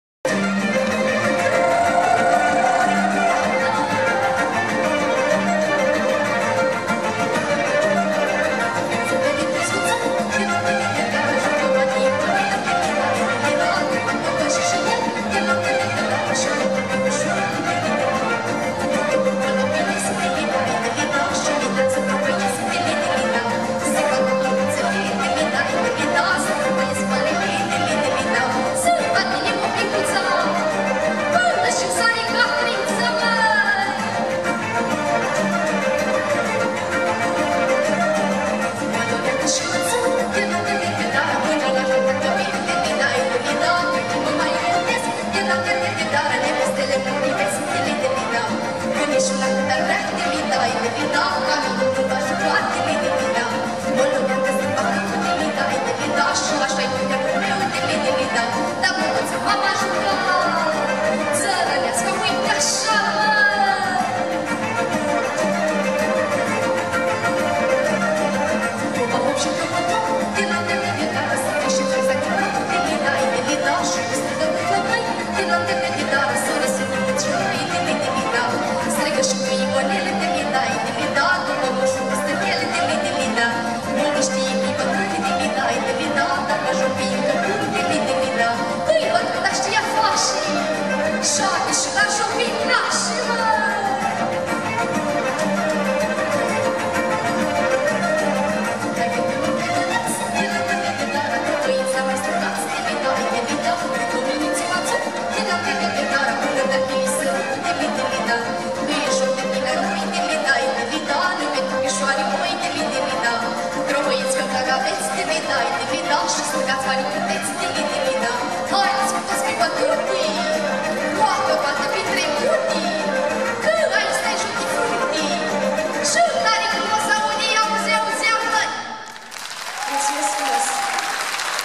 CLAUDIA TAMAS - Bacau, 26 Octombrie 2019
In background: Ansamblul Folcloric MUGURASII-Agas.
Festivalul de folclor ZESTREA - editia-a-VI-a.
Sala Teatrului de Vara "Radu Beligan” din Bacau.
(videoHD: SilvianR *** Bacau - 26 Octombrie 2019 ***).